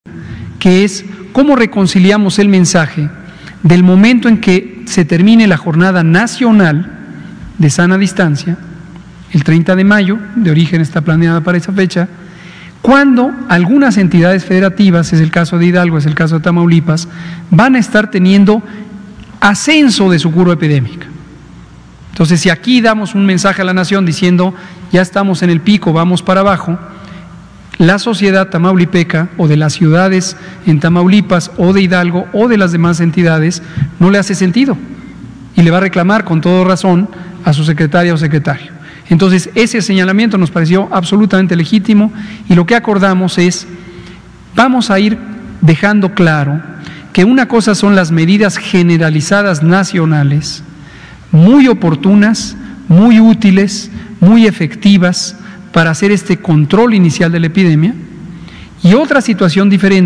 Hidalgo tendrá una cuarentena más larga
Hidalguenses tendrán que seguir en cuarentena después del 30 de mayo, curva de contagios estará en ascenso.

Hugo López Gatell, explica aquí el porque